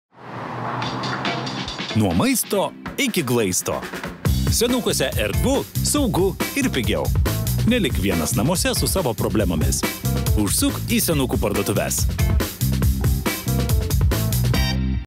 Senukai | Nuo maisto iki glaisto
Būtiniausių prekių rasi ir Senukuose. Erdvu, saugu, tik tai, kas būtina ir pigiau. Senukai. Nuo patarimo iki sprendimo.